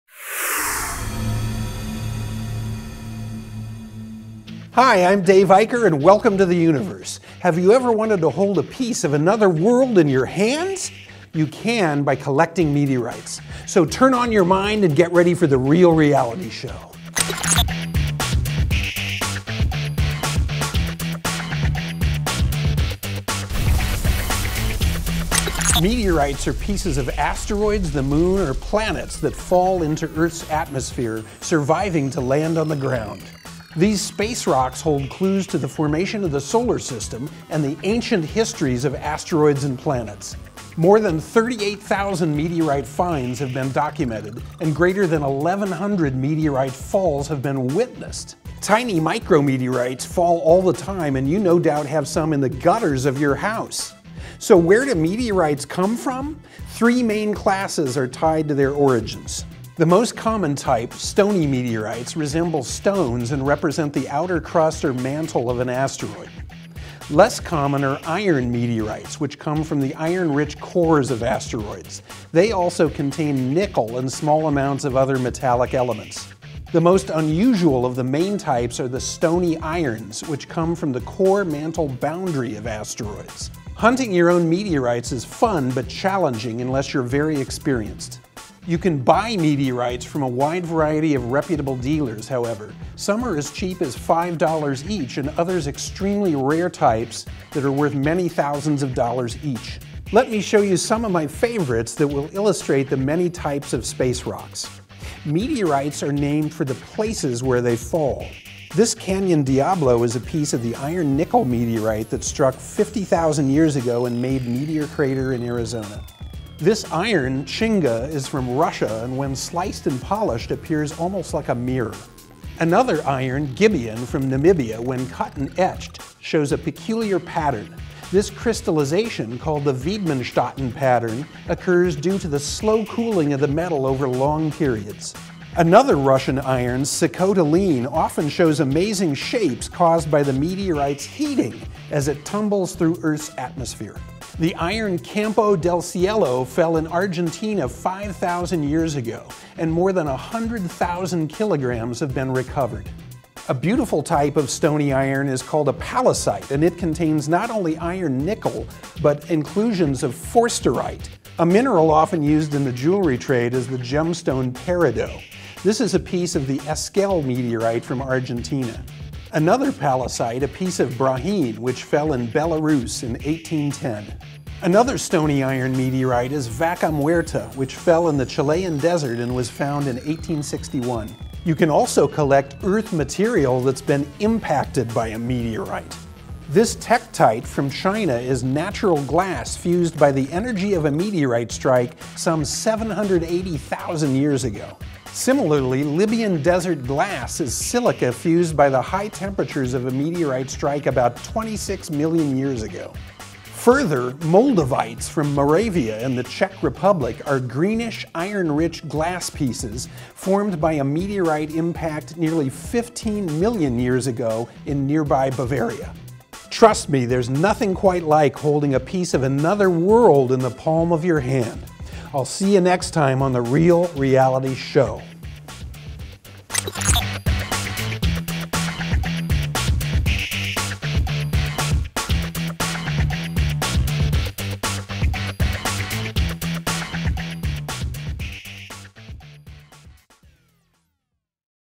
The Real Reality Show: Stones From the Sky: Collecting Meteorites
Collecting meteorites lets you hold a piece of a space rock in your hand. Join Astronomy Editor Dave Eicher to explore more types of stones from space, including rare pieces of the Moon and Mars.